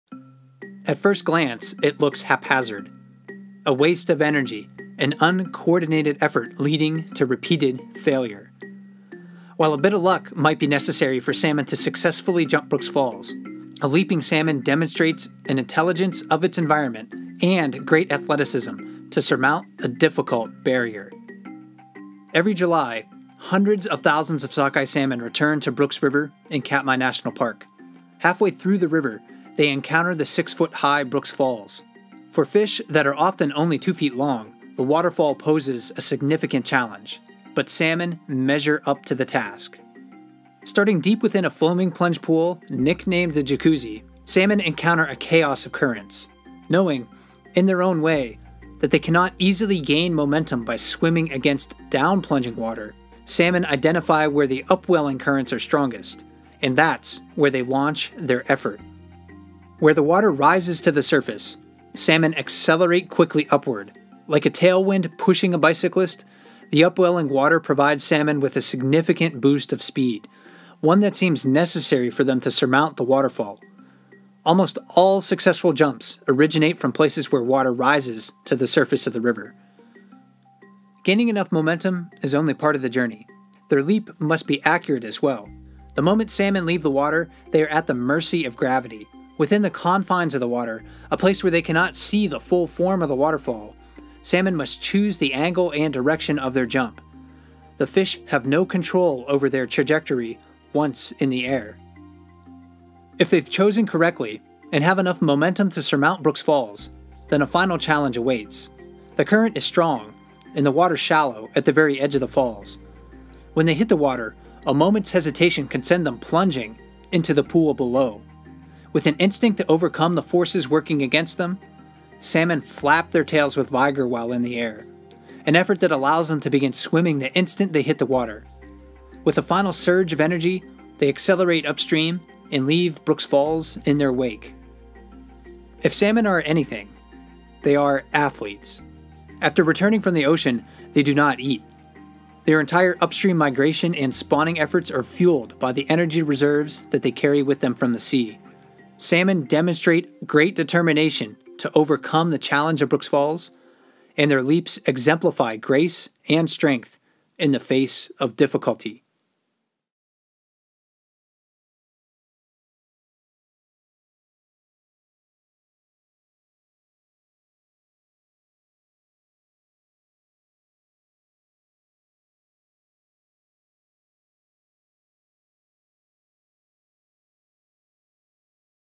How Do Salmon Jump Brooks Falls? | Mike Fitz Explores
Who else is in awe of these creatures?